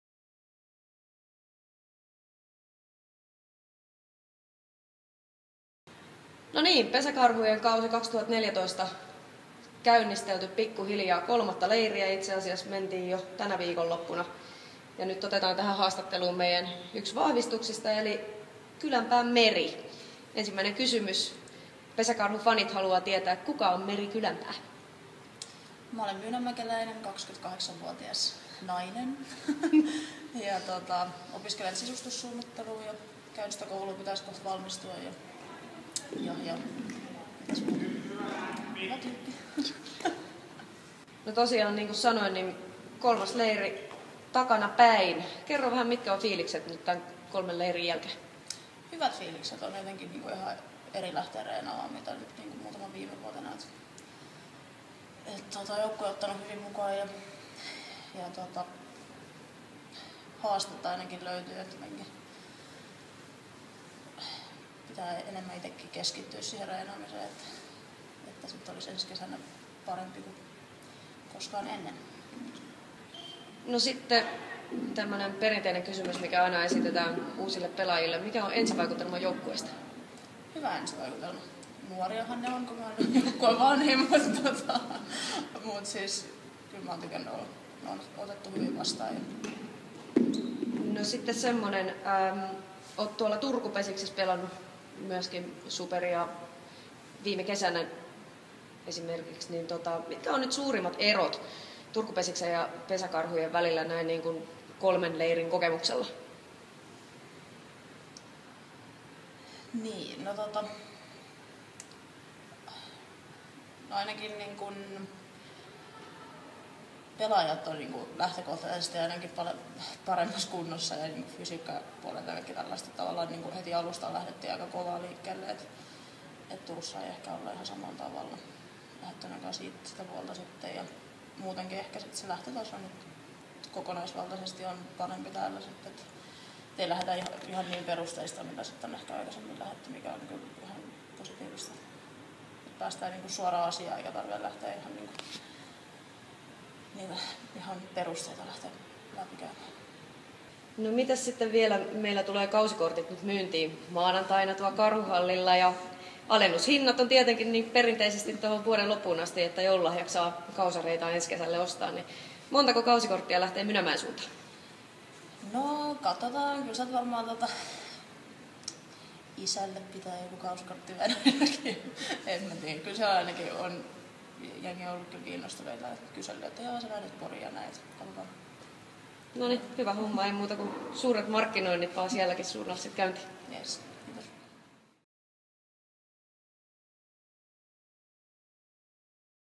Leirivideo 2 (kausi 2014)
Kuuntele Pesäkarhujen uuden vahvistuksen Meri Kylänpään mietteet leiristä, joukkueesta ja kausikorteista!